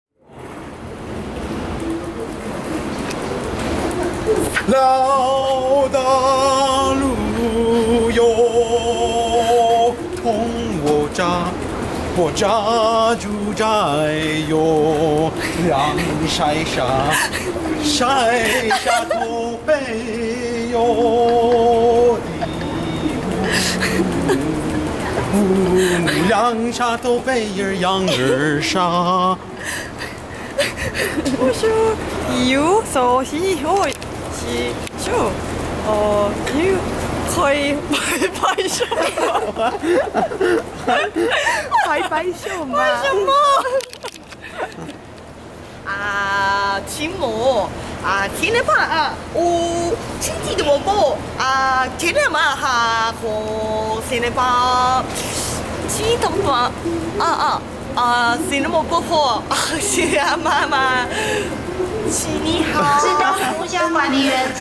Karaoke in Train Station - Taiwain
Singing is fun and you can do it anywhere you want!

Whether it be a bar or a train station, do what makes you happy!